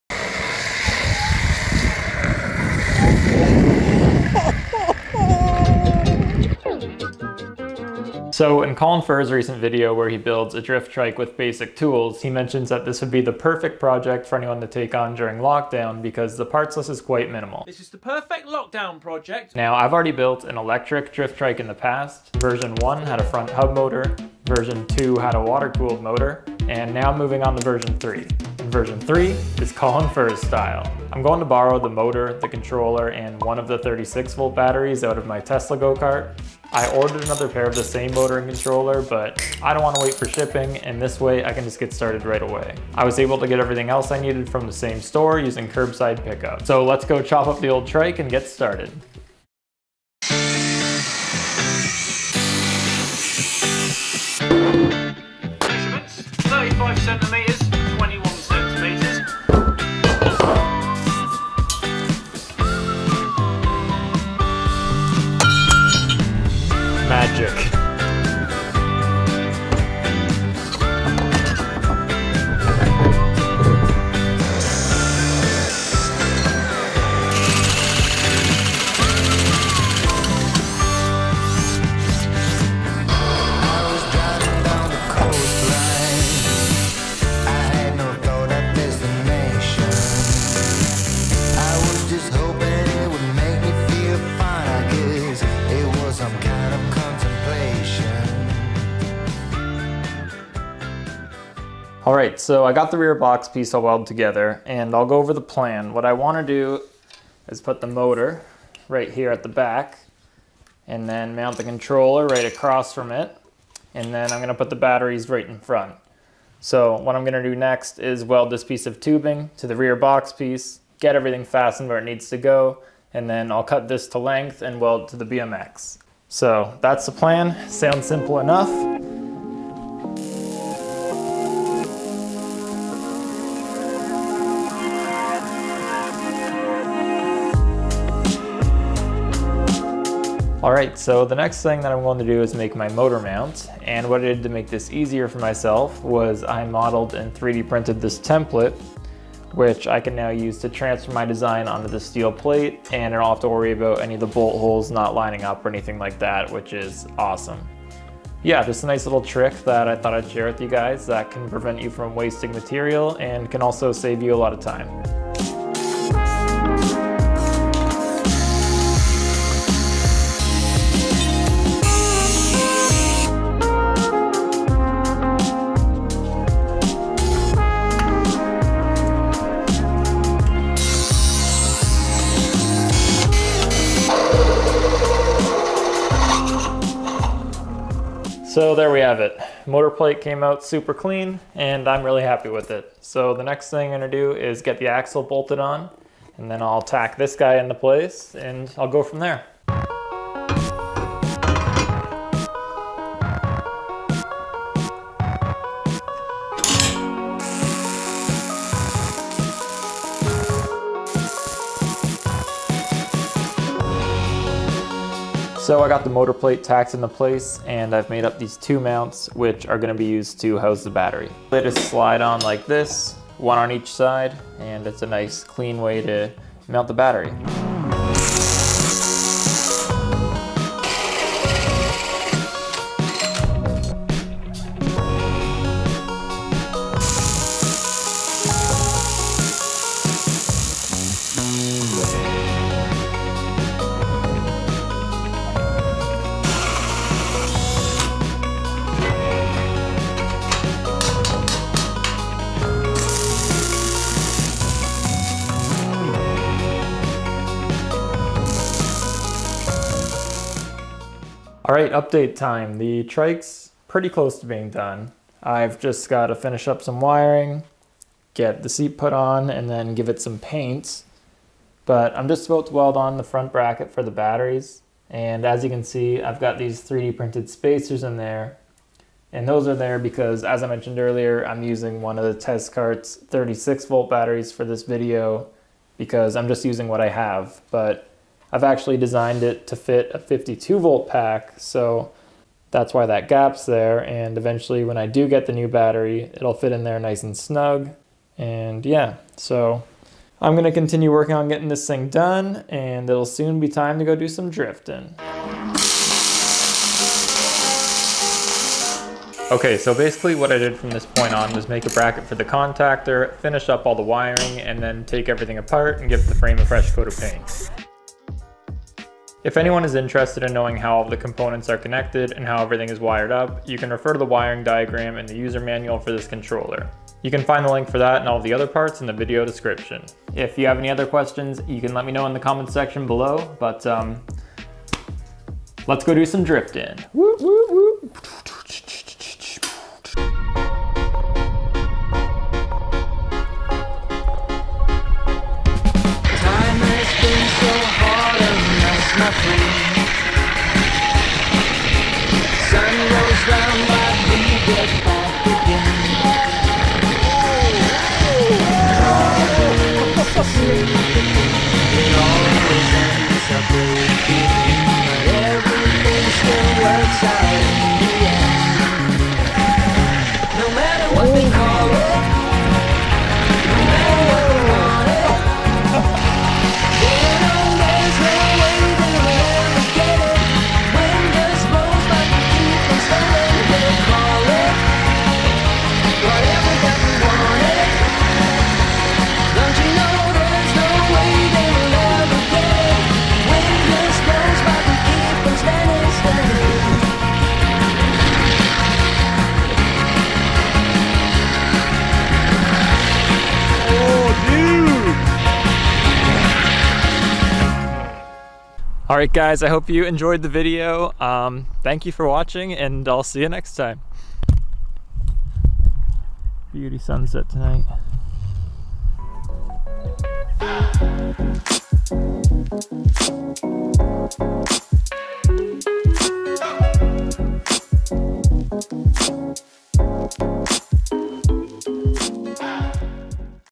Making an Electric Drift Trike (COLIN FURZE STYLE)
In this video I build a “Colin Furze” inspired electric drift trike. I tried to mostly use parts from my existing drift trike and other parts that I already had in the shop. I know this project is a little expensive and it might be tough for you to get some of the parts during lock down but I hope that I am at least able to entertain you and give you all some inspiration to build something similar one day! I would love to build a more affordable version sometime soon.

I designed the trike to house a 52V battery. Turns out 36V is more than enough power and that is all you need! The battery is arranged in a 10S1P configuration and has a 150A BMS. The sprocket ratio is 2:1. The motor sprocket has 13 teeth and the axle sprocket has 26 teeth.

BMS (150A Common)

You can find all of the other parts in my previous drift trike videos.